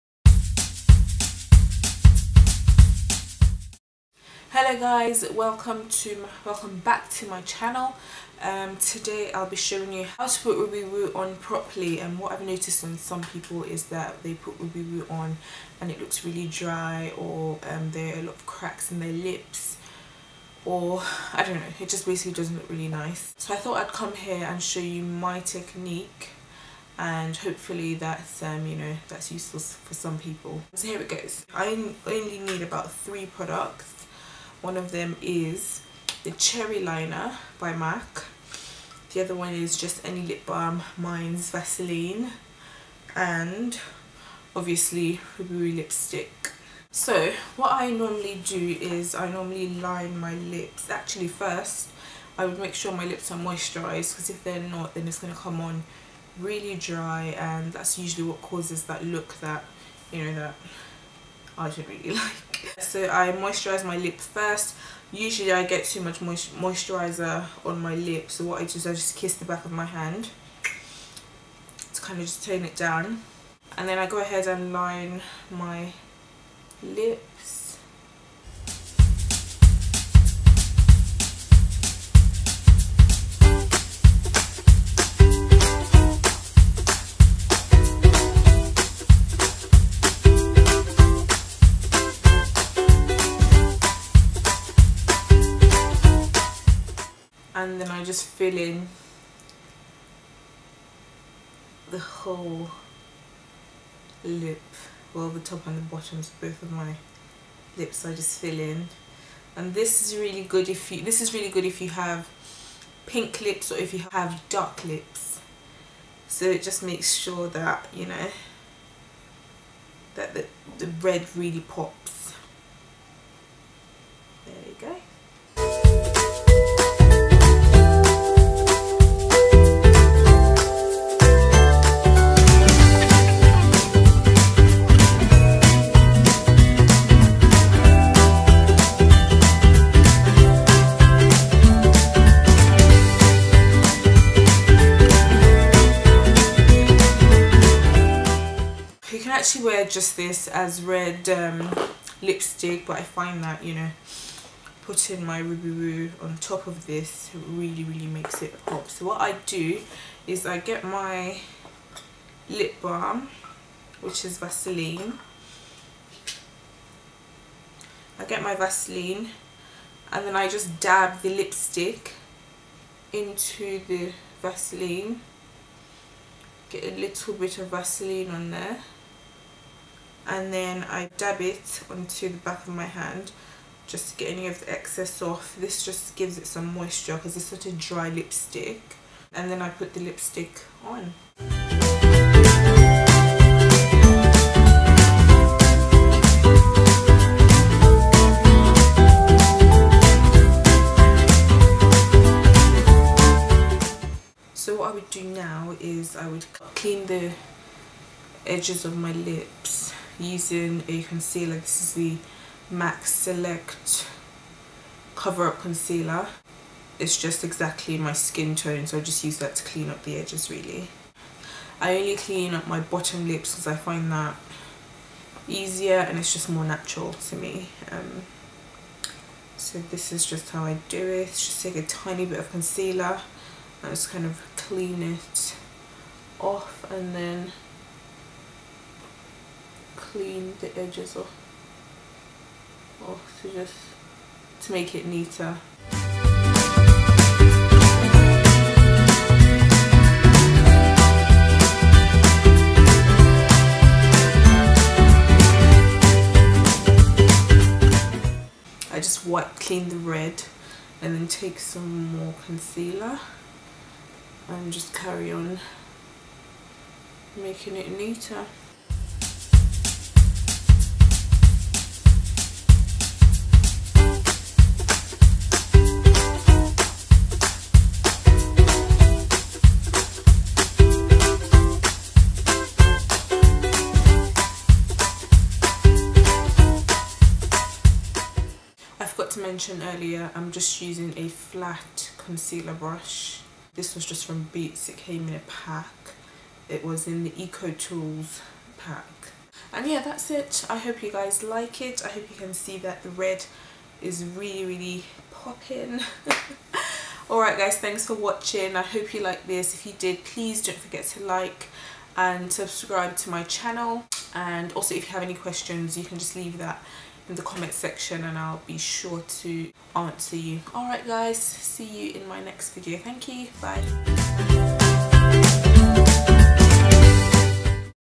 How to SLAY Red Lipstick | RubyWoo
Learn how to make your classic red lipstick (RubyWoo) POP! Its very simple!

Product List

RubyWoo Lipstick by MAC

Cherry Lip Liner by MAC

Select Cover Up Concealer by MAC

Lip balm by Vaseline